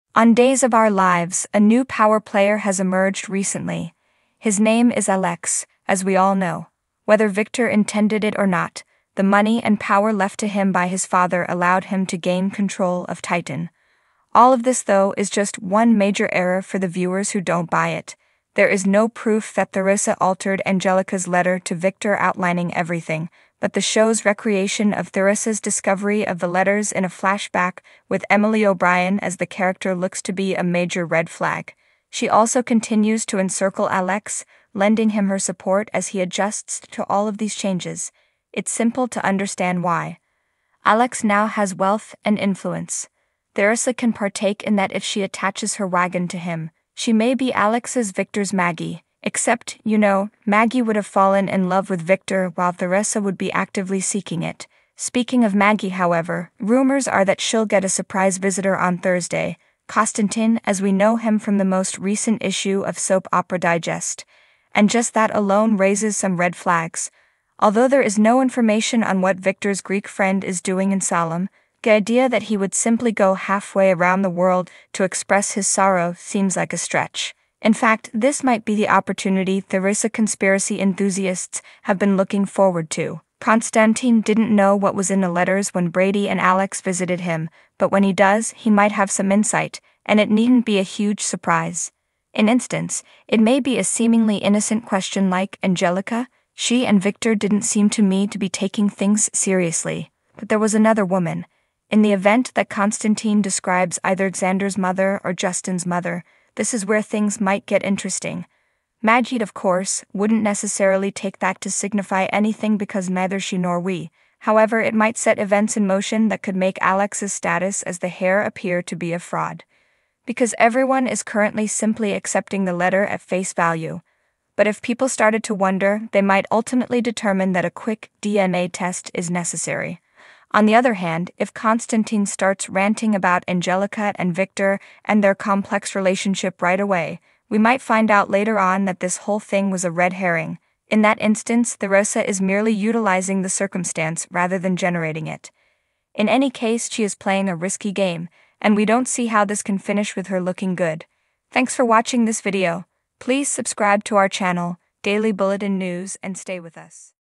Theresa Faces the Unexpected: DAYS' Shocking Comeback That Could Change Everything Prepare for Chaos
Theresa's Plan Might Not Work if The Days of Our Lives Returns.